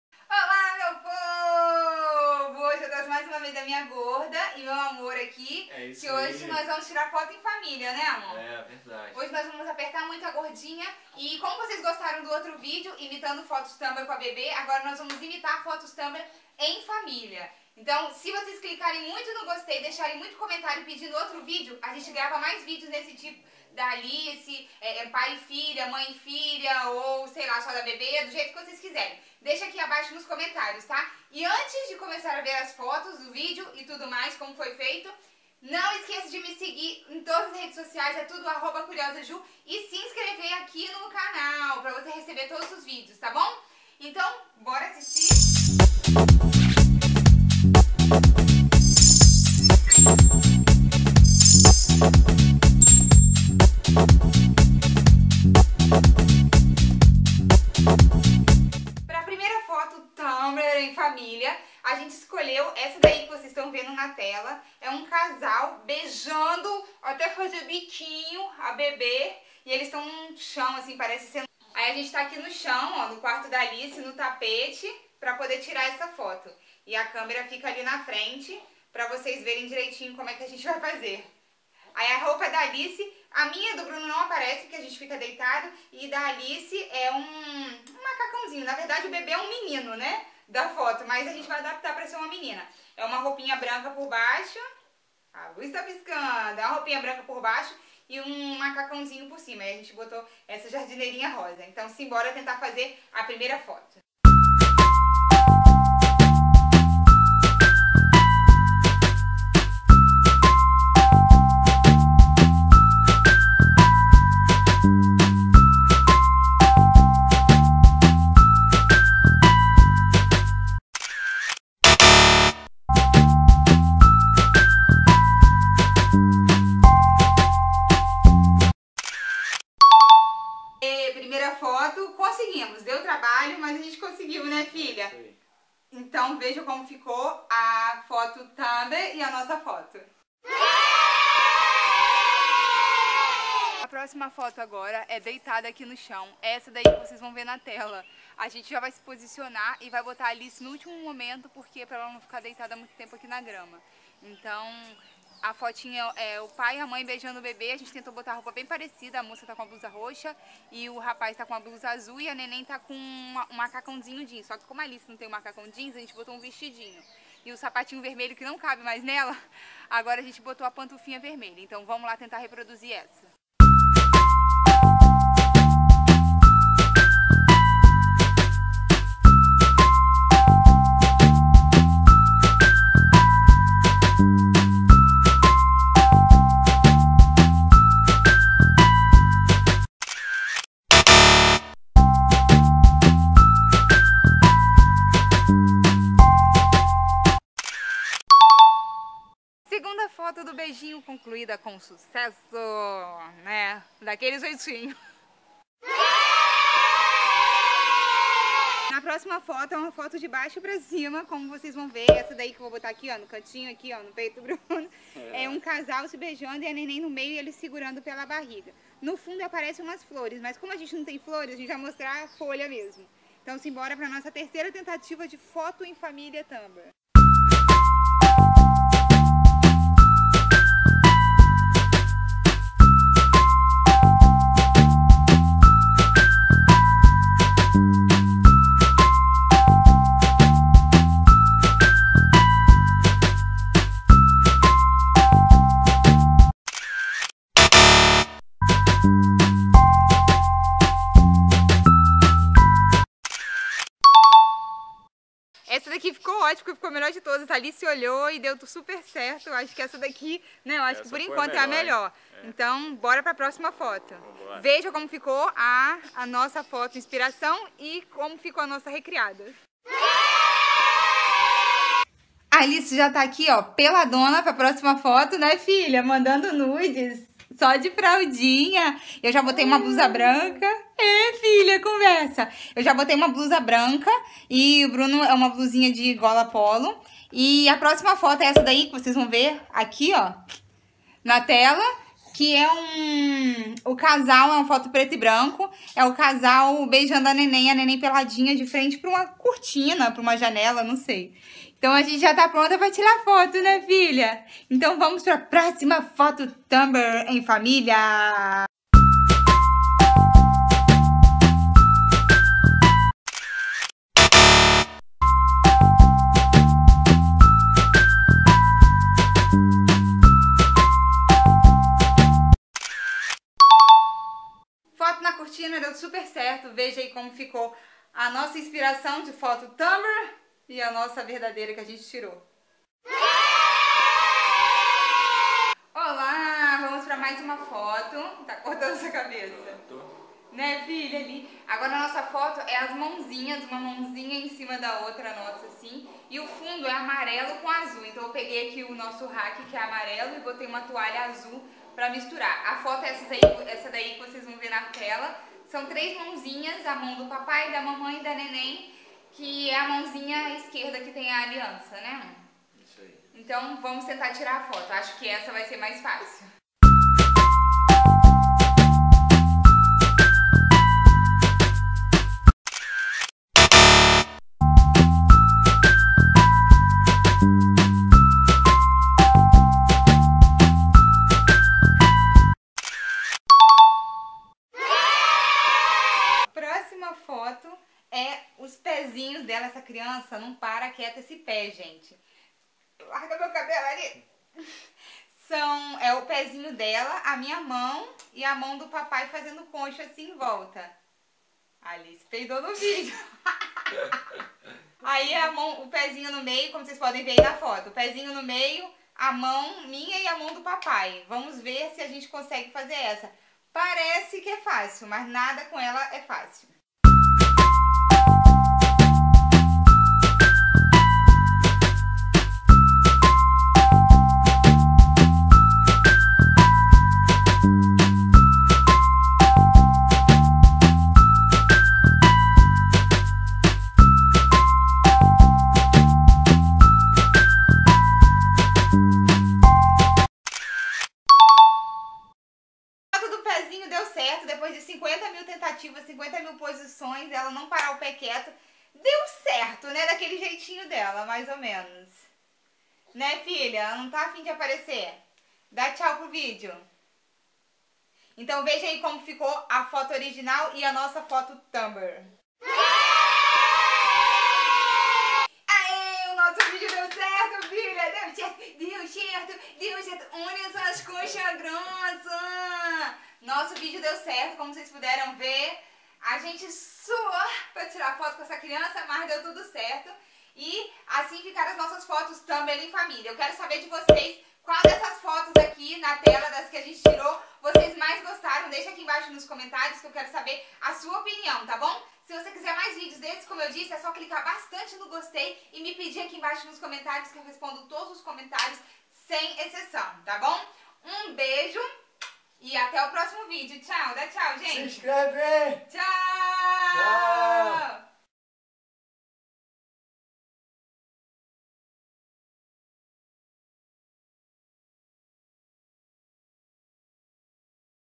Imitando FOTOS TUMBLR de BEBÊ - em FAMILIA
Entrei na onda dos vídeos da moda imitando fotos tumblr
Dessa vez trouxe a bebe Alice e o marido para me ajudarem no vídeo. Quer aprender a tirar fotos de bebe lindas em casa? Ou um ensaio com fotos em familia em casa? Então vem!